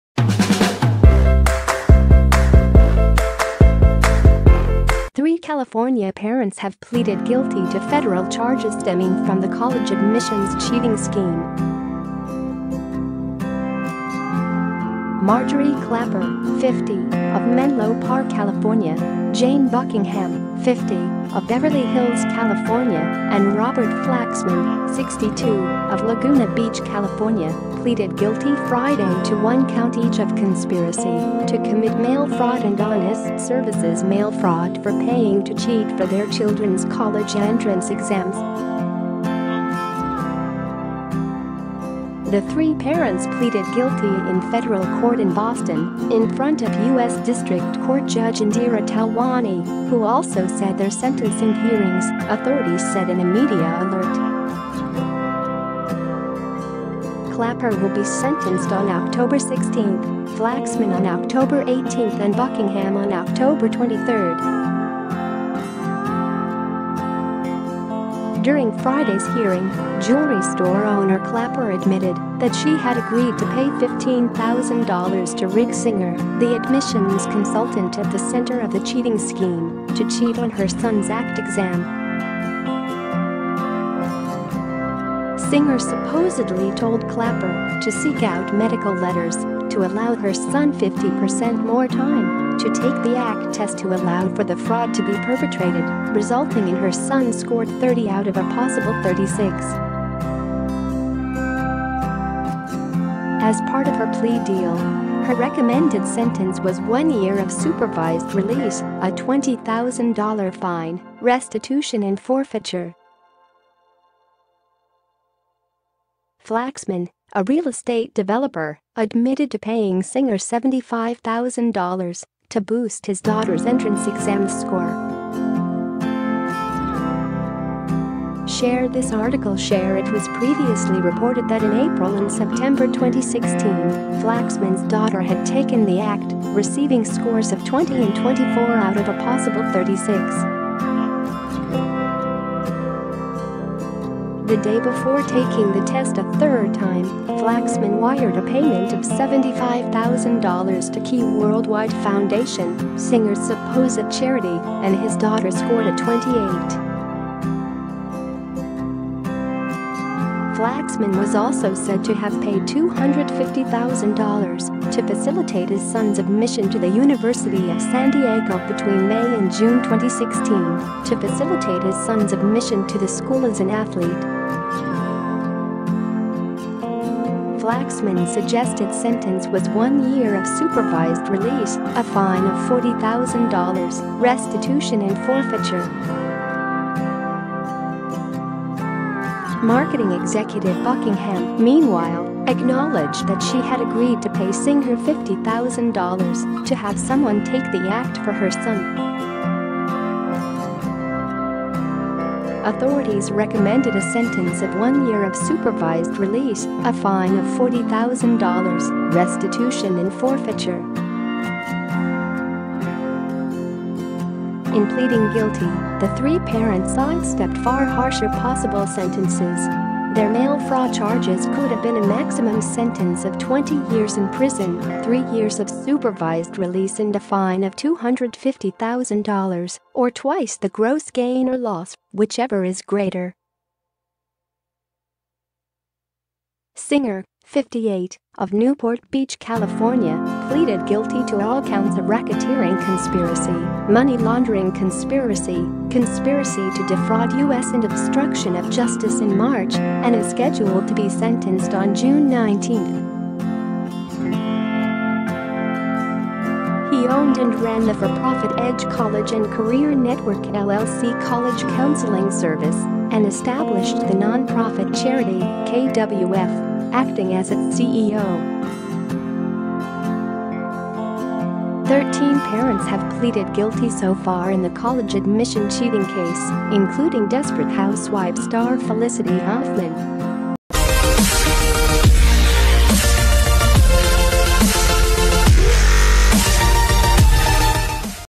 Three California parents plead guilty in college cheating scheme
Three California parents have pleaded guilty to federal charges stemming from the college admissions cheating scheme. Marjorie Klapper, 50, of Menlo Park, California; Jane Buckingham, 50, of Beverly Hills, California, and Robert Flaxman, 62, of Laguna Beach, California, pleaded guilty Friday to one count each of conspiracy to commit mail fraud and honest services mail fraud for paying to cheat for their children's college entrance exams. The three parents pleaded guilty in federal court in Boston, in front of U.S. District Court Judge Indira Talwani, who also set their sentencing hearings, authorities said in a media alert.    Klapper will be sentenced on October 16, Flaxman on October 18 and Buckingham on October 23. During Friday's hearing, jewelry store owner Klapper admitted that she had agreed to pay $15,000 to Rick Singer, the admissions consultant at the center of the cheating scheme, to cheat on her son's ACT exam. Singer supposedly told Klapper to seek out medical letters to allow her son 50 per cent more time to take the ACT test to allow for the fraud to be perpetrated, resulting in her son scored 30 out of a possible 36. As part of her plea deal, her recommended sentence was one year of supervised release, a $20,000 fine, restitution and forfeiture. Flaxman, a real estate developer, admitted to paying Singer $75,000 to boost his daughter's entrance exam score.   Share this article   Share   It was previously reported that in April and September 2016, Flaxman's daughter had taken the ACT, receiving scores of 20 and 24 out of a possible 36. The day before taking the test a third time, Flaxman wired a payment of $75,000 to Key Worldwide Foundation — Singer's supposed charity — and his daughter scored a 28.Flaxman was also said to have paid $250,000 to facilitate his son's admission to the University of San Diego between May and June 2016, to facilitate his son's admission to the school as an athlete. Flaxman's suggested sentence was one year of supervised release, a fine of $40,000, restitution and forfeiture.Marketing executive Buckingham, meanwhile, acknowledged that she had agreed to pay Singer $50,000 to have someone take the ACT for her son. Authorities Recommended a sentence of one year of supervised release, a fine of $40,000, restitution and forfeiture.In pleading guilty, the three parents sidestepped far harsher possible sentences.  Their mail fraud charges could have been a maximum sentence of 20 years in prison, three years of supervised release and a fine of $250,000 or twice the gross gain or loss, whichever is greater.  Singer, 58, of Newport Beach, California, pleaded guilty to all counts of racketeering conspiracy, money laundering conspiracy, conspiracy to defraud US and obstruction of justice in March and is scheduled to be sentenced on June 19. He owned and ran the for-profit Edge College & Career Network LLC college counseling service and established the non-profit charity, KWF, acting as its CEO.  Thirteen parents ha